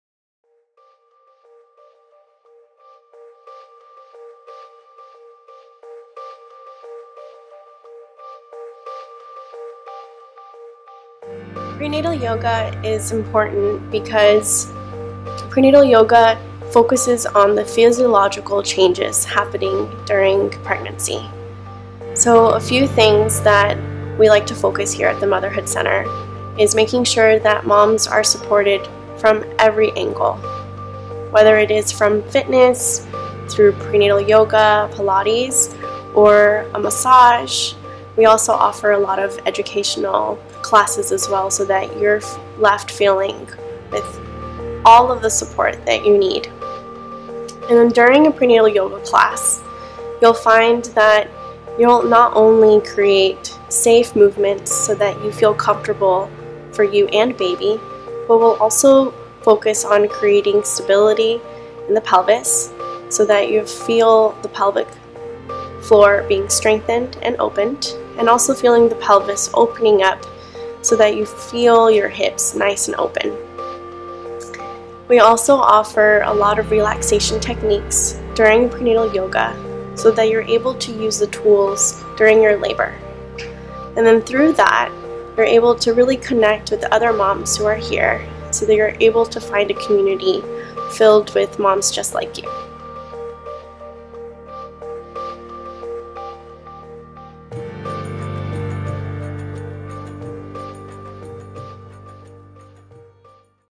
Yoga at Motherhood Center
Prenatal yoga focuses on the physiological changes happening during pregnancy. So a few things that we like to focus here at the Motherhood Center is making sure that moms are supported from every angle, whether it is from fitness through prenatal yoga, pilates, or a massage.